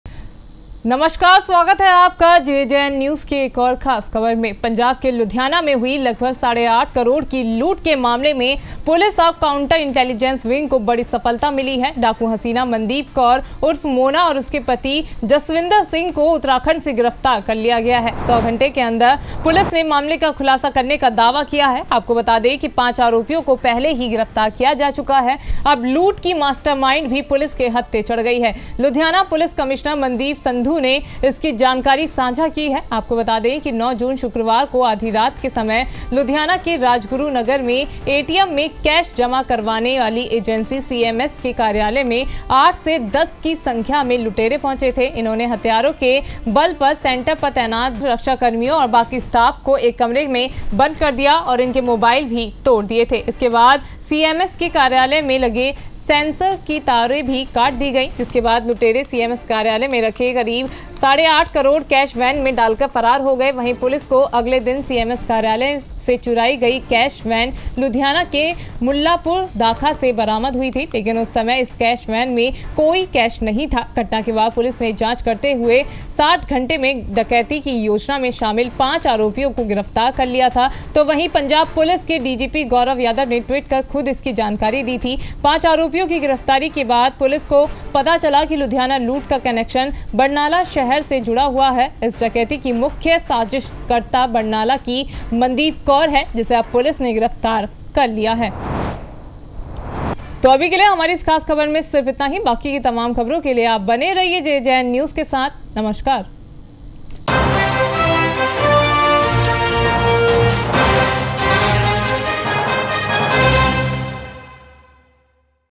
साढ़े 8 करोड़ की लूट की मास्टरमाइंड डाकू हसीना उत्तराखंड से गिरफ्तार || JJN News
JJN News Nainital,
JJN News,